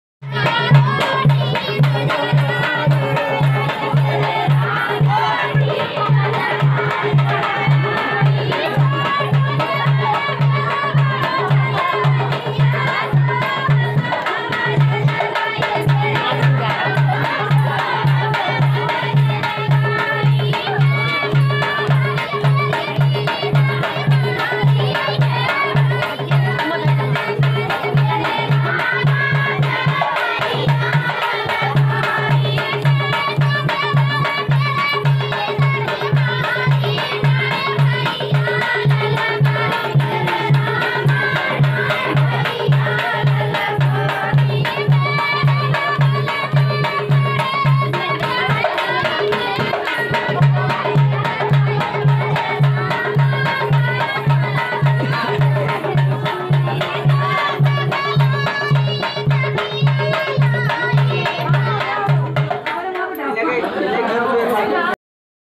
सभी महिलाओं का ऐलान किया धमाकेदार खोइया फौजिन चाची का गजब डांस एस एस यादव स्टूडियो
देहाती औरत नाच गीत
देहाती डांस
देहाती नाच